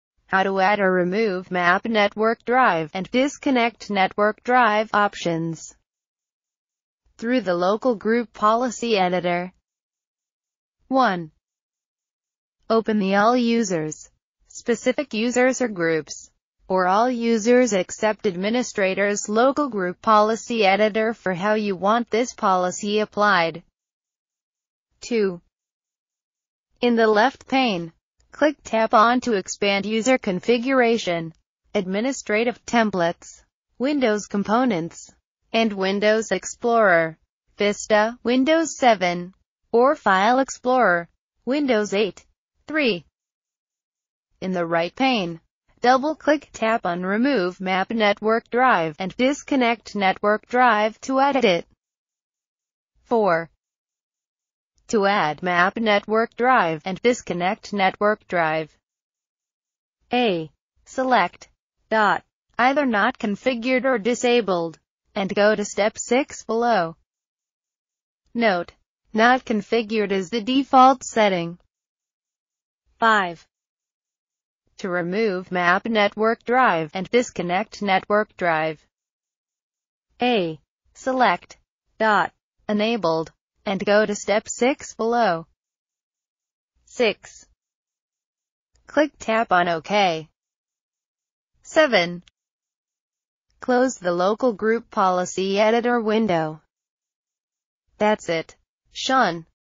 How to Add or Remove Map Network Drive and Disconnect Network Drive Options
How to Add or Remove Map Network Drive and Disconnect Network Drive Options How to Add or Remove "Map Network Drive" and "Disconnect Network Drive" Options
Through the Local Group Policy Editor
1. Open the all users, specific users or groups, or all users except administrators Local Group Policy Editor for how you want this policy applied.
2. In the left pane, click/tap on to expand User Configuration, Administrative Templates, Windows Components, and Windows Explorer (Vista/Windows 7) or File Explorer (Windows 8).
3. In the right pane, double click/tap on Remove "Map Network Drive" and "Disconnect Network Drive" to edit it.
4. To Add "Map Network Drive" and "Disconnect Network Drive"
A) Select (dot) either Not Configured or Disabled, and go to step 6 below.
NOTE: Not Configured is the default setting.
5. To Remove "Map Network Drive" and "Disconnect Network Drive"
A) Select (dot) Enabled, and go to step 6 below.
6. Click/tap on OK.
7. Close the Local Group Policy Editor window.
That's it,
Shawn